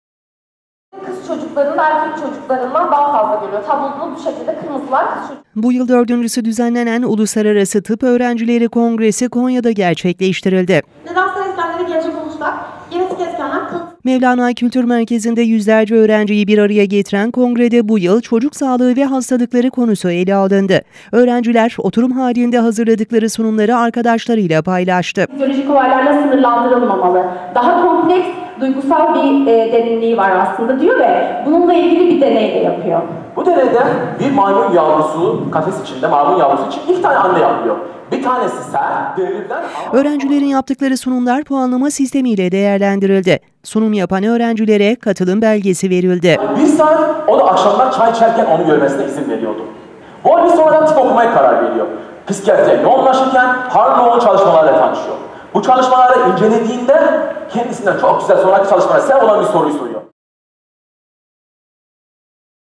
Tıp öğrencileri Konya'da buluştu
Tarhan, Selçuk Üniversitesi (SÜ) tarafından Mevlana Kültür Merkezi'nde düzenlenen 4'üncü Avıcenna Uluslararası Tıp Öğrenci Kongresinde, "X, Y, Z Kuşağı ve İletişimde Zorluklar" konusunda genç tıpçılara tavsiyeler verdi.